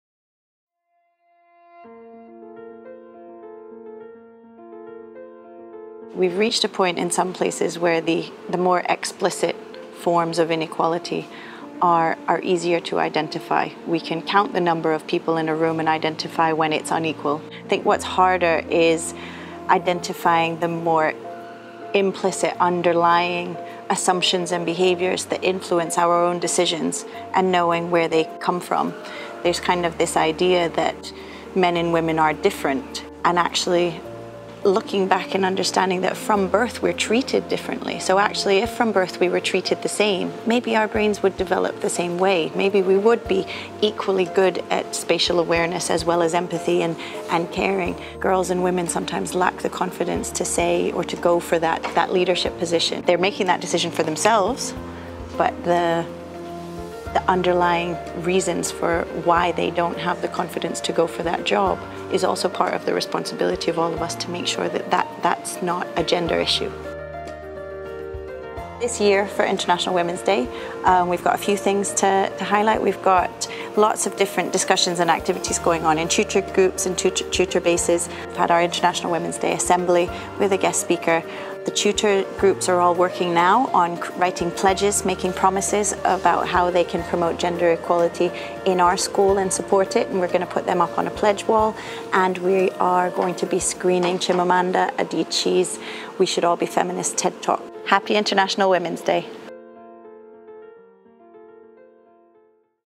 Gender Equality Through The Lens of Ainslee Dann
"Girls and women sometimes lack the confidence, to say or to go for that leadership position. They're making decision for themselves, but the underlying reasons for why they don't have the confidence to go for that job is also part of the responsibility of all of us to make sure that is not a gender issue." - Ainslee Dann, Nexus Director of Curriculum Enhancement.

Find out more about gender equality through the lenses of our Nexus Community in this video. Happy International Women's Day.